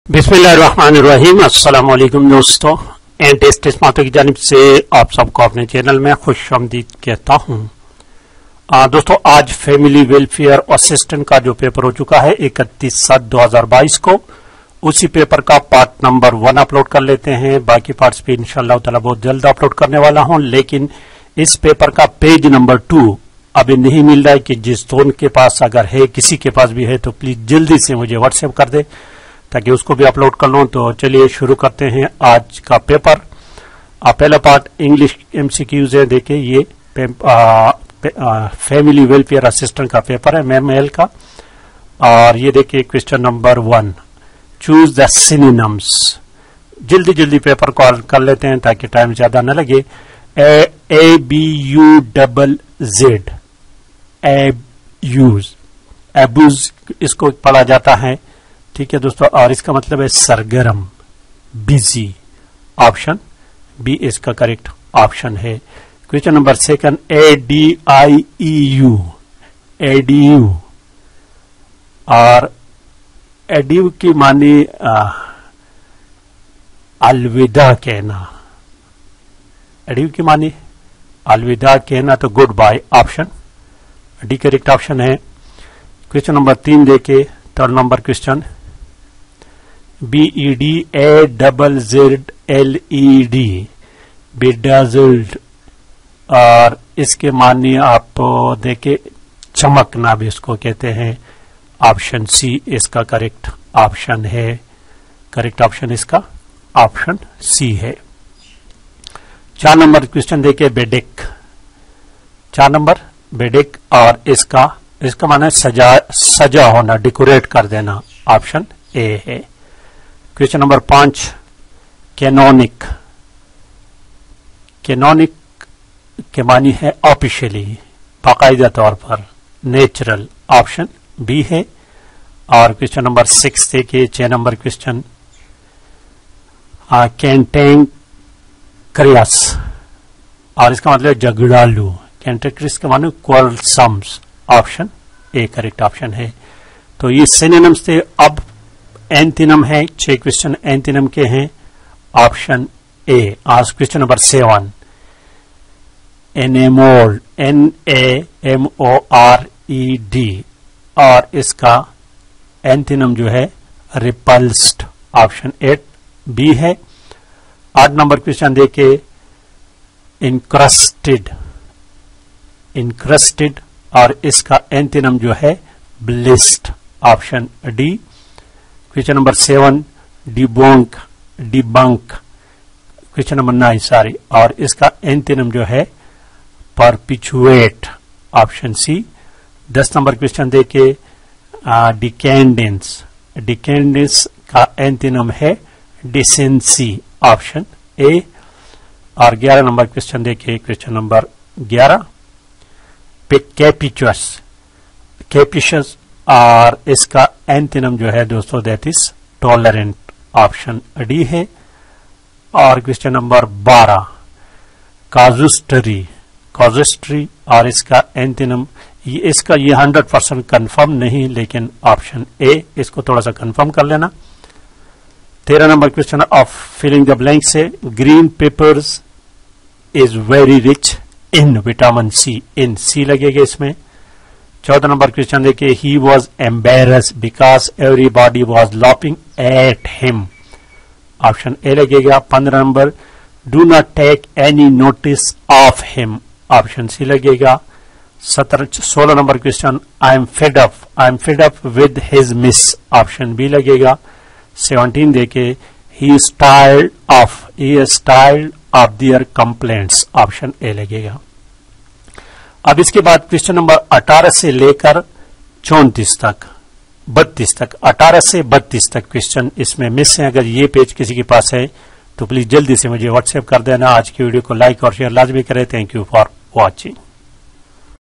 ATS Family welfare Assistant 31-07-2022 paper : Family welfare assistant paper July 2022: part - 01
if in a certain code language similar is written as izornrh, then how will
if in a certain code language Tiger is written as GRTVT then what word will be written for HMZPV
introducing a man a woman said his wife is the only daughter of my mother
rehman and babu are twins, babu's sister is rimsha, rimsha's husband is rehan. rehan's mother i laiba. laiba's husband is riaz. how is the riaz related to rehan:
a cow is to a calf as goat is to: mcqs
doctor is related to patient in the way as lawyer is related to
Coins mint bricks
river stream ocean
a sum of Rs. 1600 gives a simple interest of Rs. 252 in 2 years and 3 months. the rate of interest per annum is: mcqs
a does half as much work as b in one sixth of the time. If together they take 10 days to complete a work, how much time shall B take to do it alone: mcqs
when a number is divided by 56 the remainder obtained is 29, what will be the remainder when the number is divided by 8: mcqs
each member of a picnic party contributed twice as many rupees as the total
if (n-1) is an odd number what are the two other odd numbers nearest to it: mcqs
if x y z be the digits of a number beginning from the left the number is
He is tired their complaints. mcqs
I am fed up his mess: mcqs
do not take any notice – him: mcqs
he was embarrassed because everybody was laughing him: mcqs
green pepper is very rich vitamin c: mcqs
Abuzz : سرگرم
synonym for abuzz: mcqs
synonym of adieu: mcqsالوداع
synonym of bedazzled: mcqs  چمکنا
synonym of bedeck : arasta
synonym of canonic
synonym of cantankerous: jagralo
antonym of  enamored: pur joosh
antonym of encrusted : covered or decorated with a hard surface layer.
Antonym of Debunk : bayneeqab karna
Antonym of decadence: tanzulee
Antonym of captious: qeedi aseer

p and q are two positive integers such that PQ = 64. Which of the following cannot be the value of  P + Q:  (1,64),(2,32),(4,16) and (8,8)
how many numbers between 1000 and 5000 are exactly divisible by 225
A alone can complete a work in 12 days. A and B together can complete it in 8 days. how long will B alone take to complete the work
which of the following is always odd: mcqs
Rs.500 was invested at 12% per annum simple interest and another certain sum of money invested at 10% per annum simple interest. If the total interest on both the sum after 4 years is Rs. 480, the latter sum of money is :

there are just two ways in which 5 may be expressed as the sum of two different positive (non-zero) integers, namely 5 = 4 + 1 = 3 + 2. in how many ways 9 can be expressed as the sum of two different positive non zero intergers
to get into hot water : mcqs
from stem to stern: mcqs
over egg the pudding means: mcqs
as me i will be happy to lend a hand: mcqs
it differs their last suggestion: mcqs
he said, he does not like beef: change the narration
the girl said i like sleeping change the narration
aslam said to me, if i hear any news, i will phone you" change the narration
she said to me, he mostly ignores me. change the narration
the teacher said to sam, congratulations! wish you success in life: change the narration
we consider the 14th of february a red letter day:
at the police station, he showed a clean pair of heels.
He brake off in the middle of the store.
antonym of cautiosly : mcqs
antonym of crotchety : mcqs
she care the environment: mcqs
it was kind you to help: mcqs
unfortunately, we had to cancel it owing the bad weather: mcqs
he was accused theft: mcqs

Synonyms of abstruse : difficult to understand : ناگوار : غیر واضح
Synonym of beady:
Antonym of endear :
antonym of debonair: mcqs
antonym of debouch: mcqs
synonym of beaver : mcqs
synonym of calumny:  لعانت
synonym of camaraderie  : mcqs dosti
antonym of encumber: mcqs  booj

Synonyms of abstinance :  پرہیز
what are the health benefits of oral contraception: mcqs
besides incorrect use, what is another reason that condoms may not be effective: mcqs
what is the most common cause of death in diarrhea: mcqs
anything that cab be used to earn a living is called : mcqs
why the hospital practices are monitored: mcqs
Pakistan’s population was only million at the time of independence;
which of the following is a prenatal test used to help identify genetic abnormalities through a maternal blood test
the clinic nurse who guides client's development in effective problem solving and decision making begins by : mcqs
the number of people/square km of land is classified as: mcqs
a well was present in a rural area when an unsanitary borehole latrine with lots of files was present with in 10 feet of distance. The disease more likely to be transitted through drinking this well water will be: mcqs